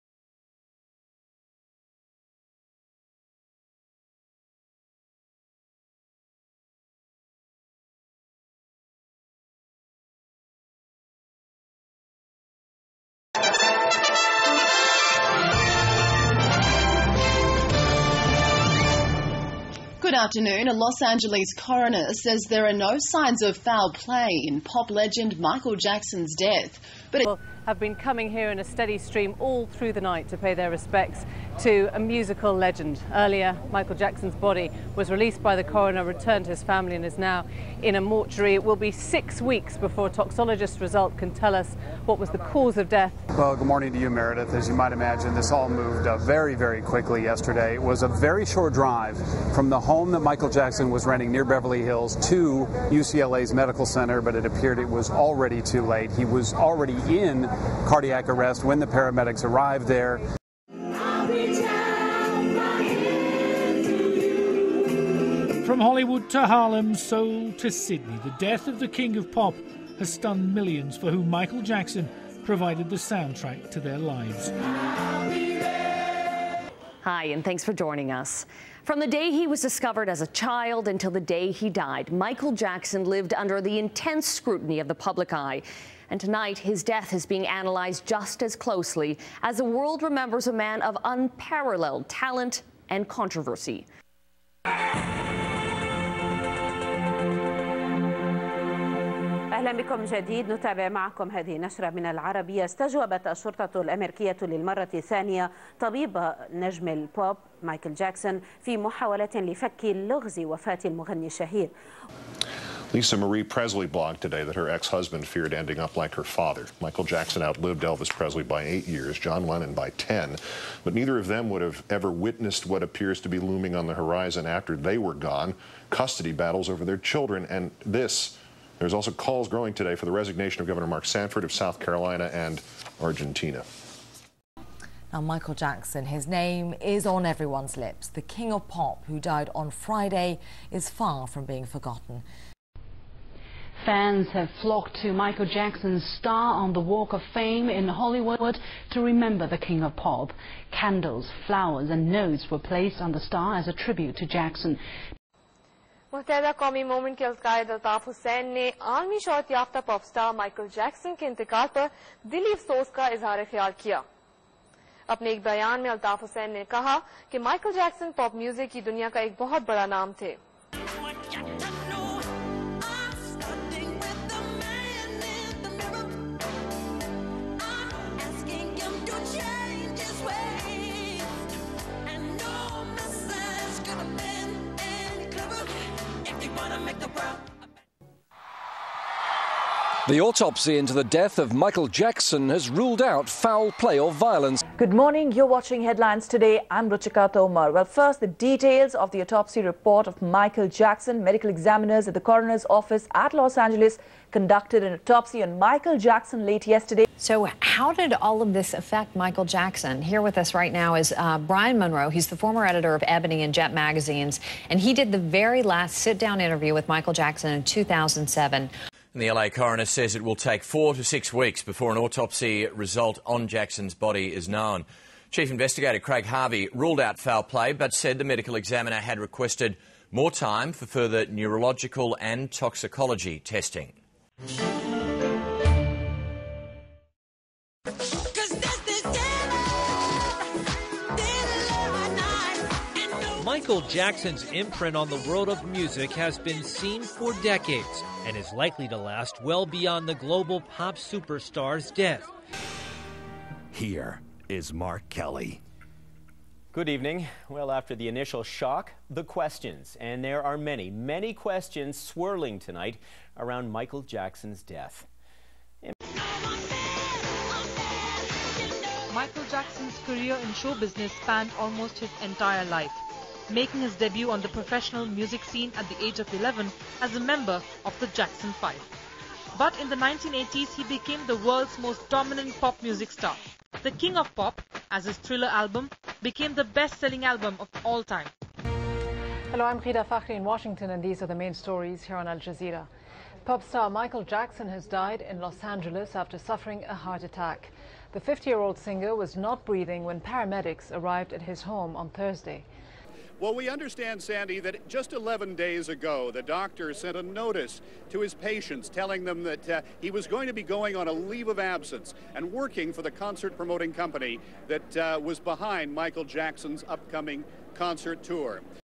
Media Coverage of Micheal Jackson's Death
On June 25 2000 Michael Jackson died.  Jackson was one of the most well-known celebrities and that was reflected in the massive amount of media coverage he received.  From Beijing to Moscow, Sydney to Dubai Jackson's death was headline news.